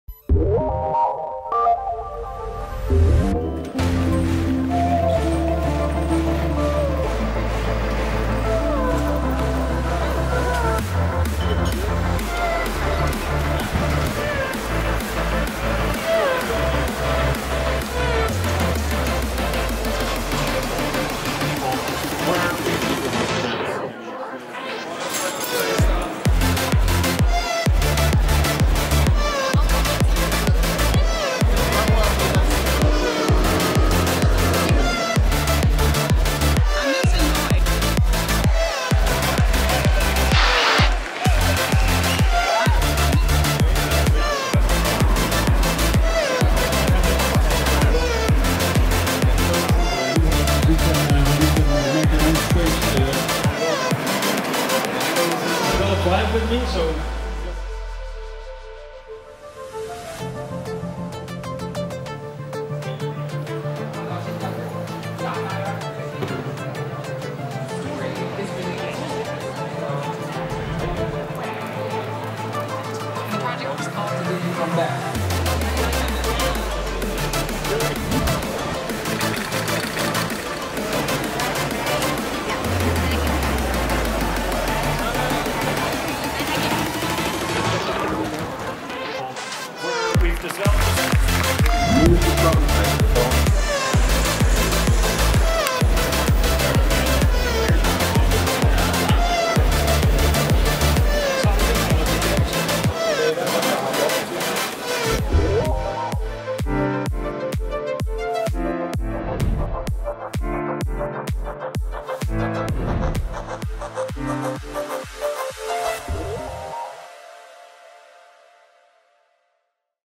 Campus Party Europe in The Netherlands
Campus Party is the greatest technological experience of the world which brings together young geeks & aspiring entrepreneurs in a festival of innovation, creativity, science, digital entertainment & entrepreneurship.

The event was held in Utrecht between May 25 - 29 / 2016

 Made by Campuseros Machiel Kastelein & Paul Eggink